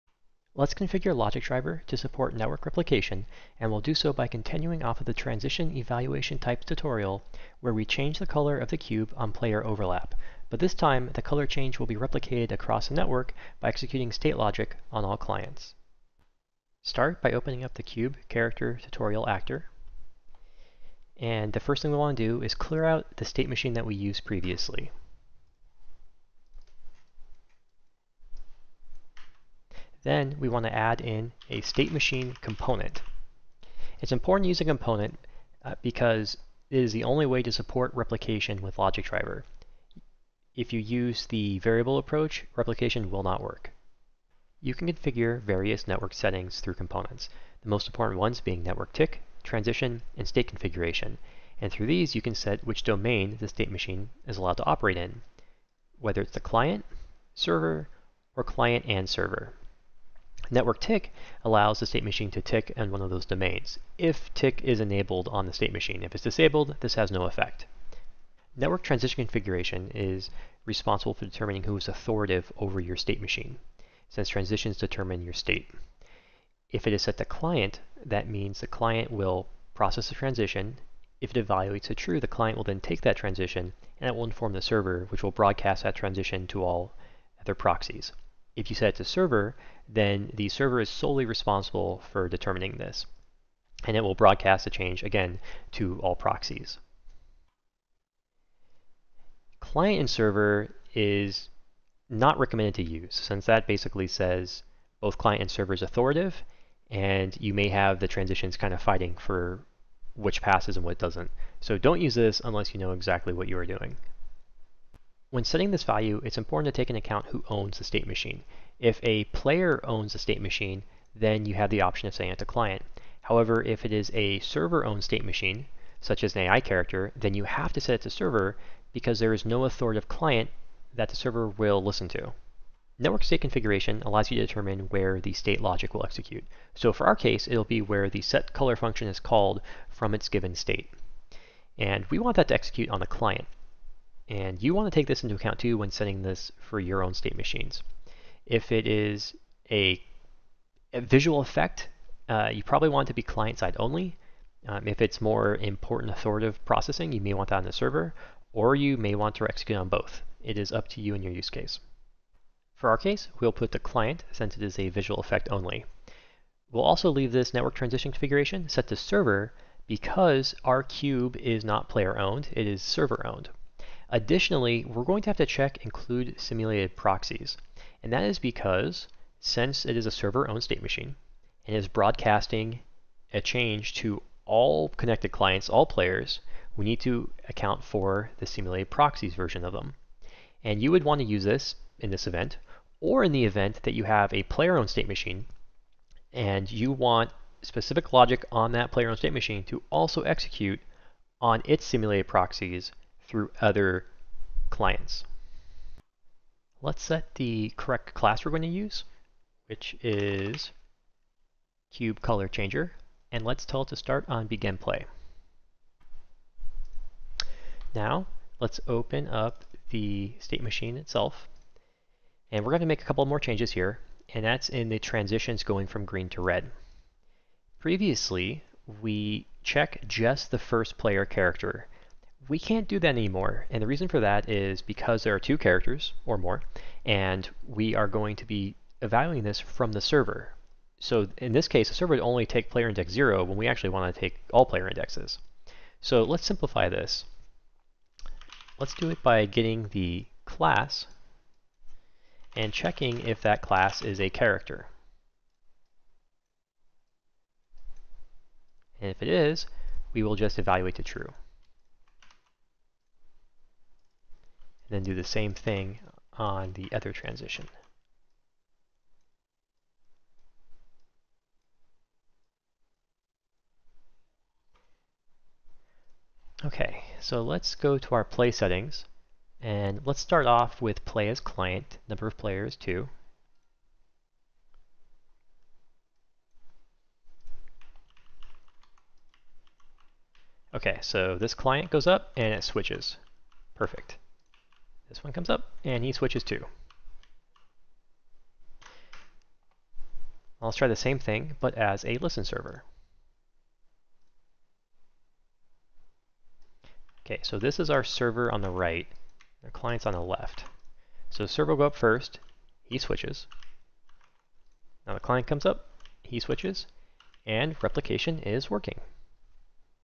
Logic Driver Tutorial: Intro to Networking
Use a state machine to change the color of a cube through replication.

0:00 Setup
0:44 Network Settings Overview
2:42 Configure Replication
3:38 Reconfigure Transitions
4:36 Test Replication